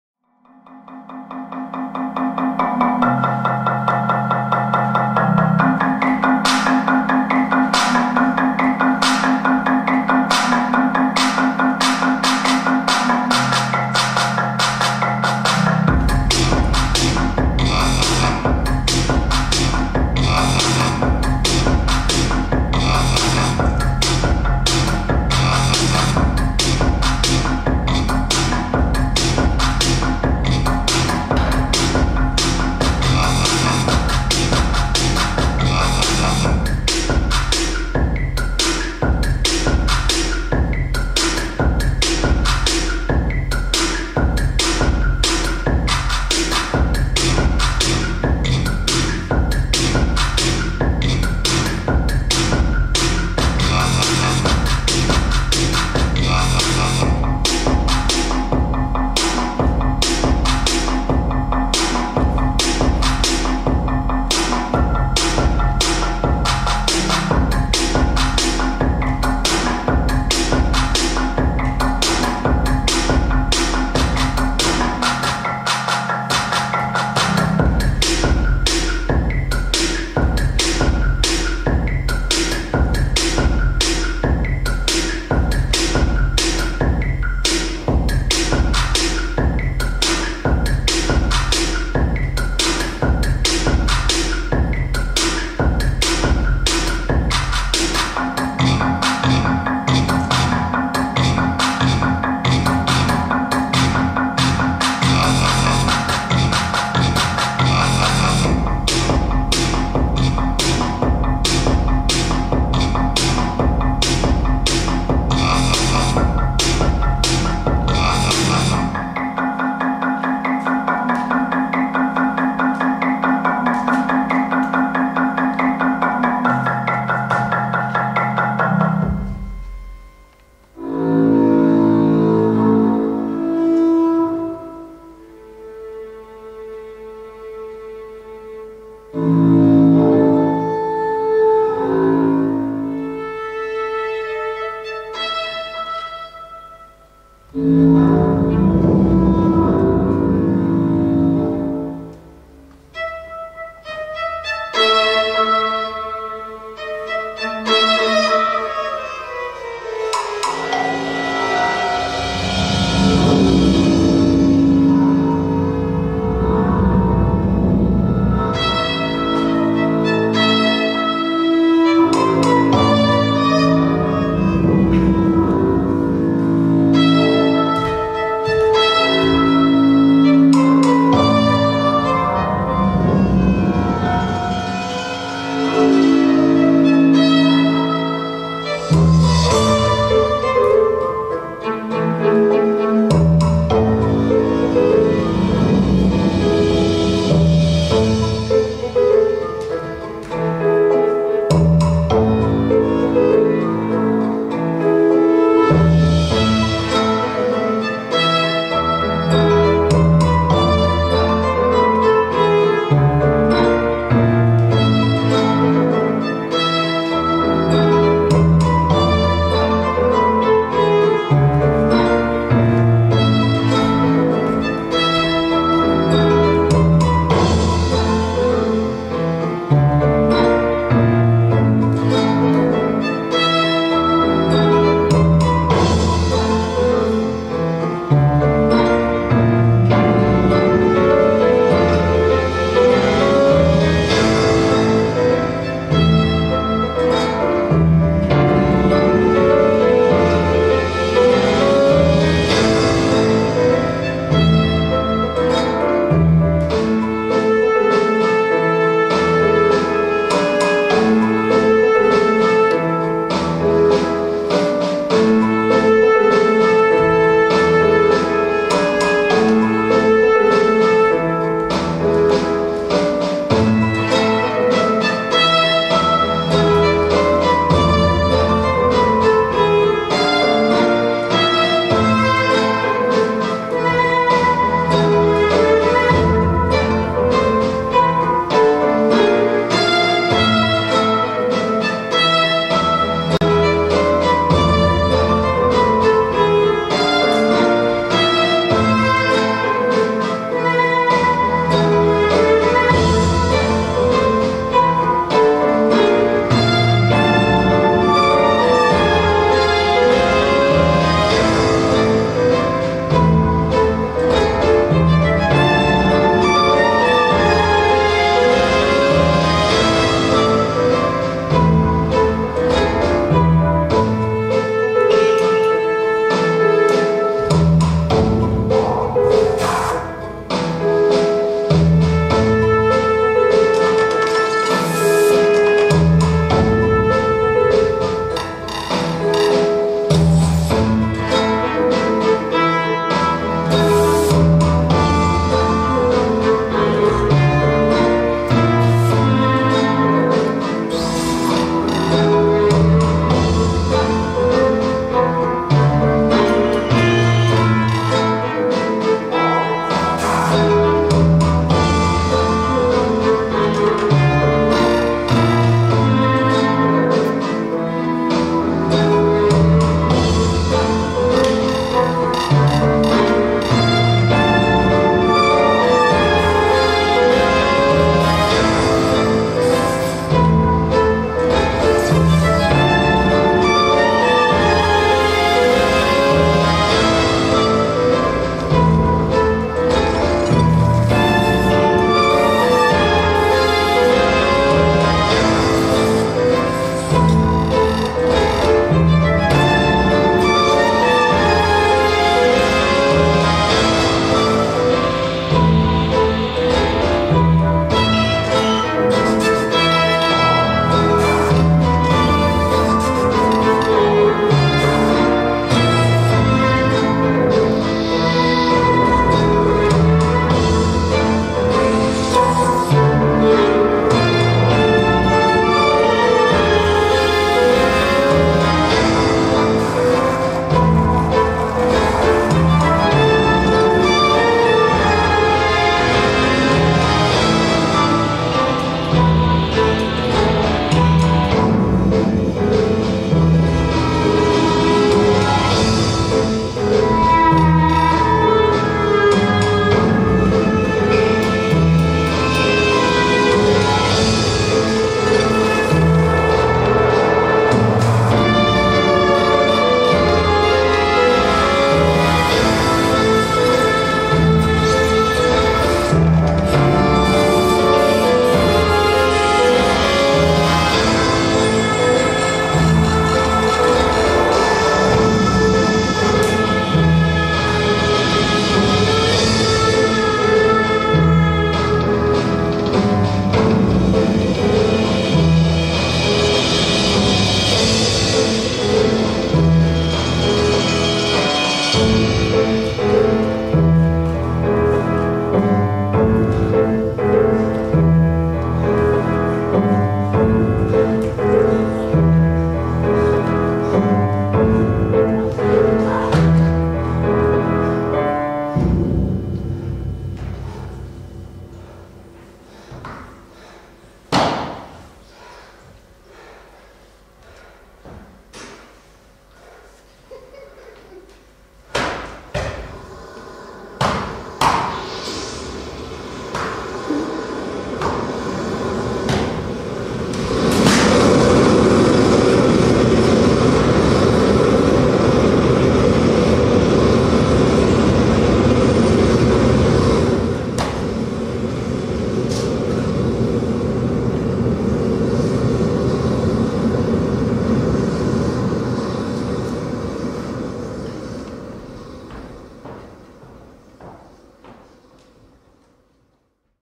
kismet
triet created by Loco:Motion Company, performing: Katerina Eva Lanci, Vaclav Kovar, Matej Kohout, Sound and music by Adam Lanci, Light design Jan Dluhos,
piece is a part of performance called Kismet - irreversible destiny. Premiera in april 2012 in Synagogue in Libeň, Prague, Czech Republic